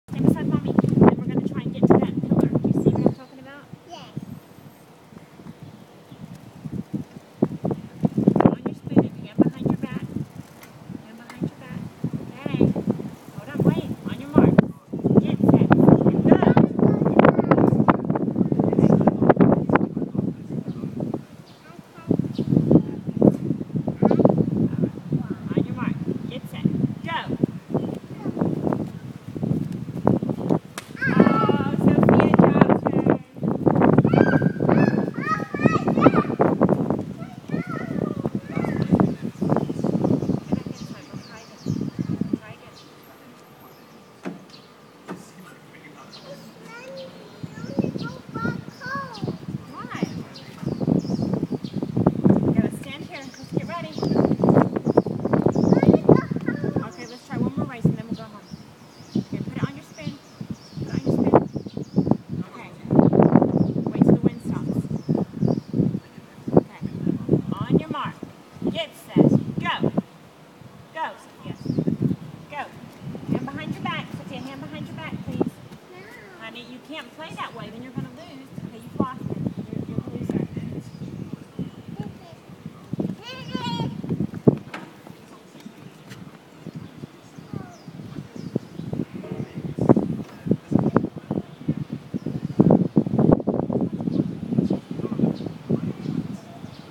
Egg and spoon racing
Sophia and moma at the park trying the new egg and spoon kit.